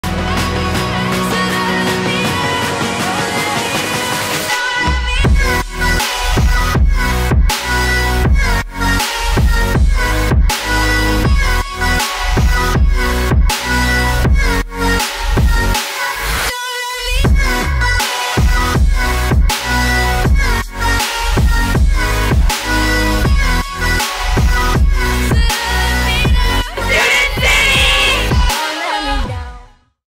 StudentCity South Padre Island, Texas Spring Break 2016
StudentCity changed the game in South Padre Island, Texas this Spring Break! We hosted the Isla Grad Beach Bash with talent such as Lil Dicky, The Chainsmokers, and Lil Jon, as well as the best pool parties and night events on the island. Be a part of it next year!